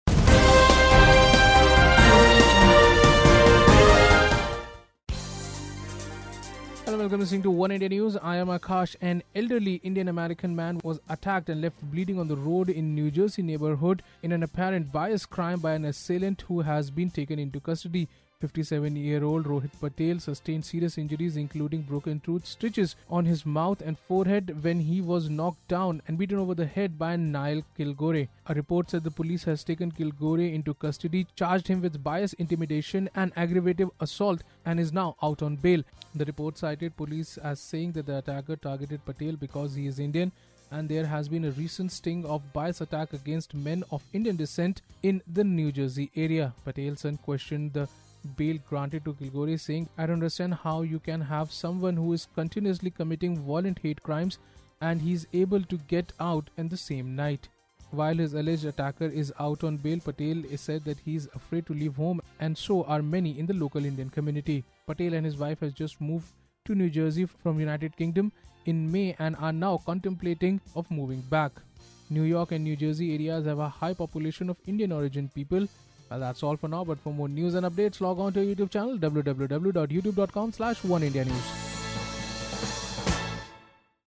New Jersey Attack: Elderly Indian beaten, attacker out on Bail
An elderly Indian-American man was attacked and left bleeding on the road in a New Jersey neighborhood in an apparent bias crime by an assailant who has been taken into custody. Rohit Patel, 57, sustained serious injuries including broken teeth, stitches on his mouth and forehead when he was knocked down and beaten over the head by Nyle Kilgore in North Brunswick last week. A report said police had taken Kilgore, into custody, charged him with bias intimidation and aggravated assault and is now out on bail.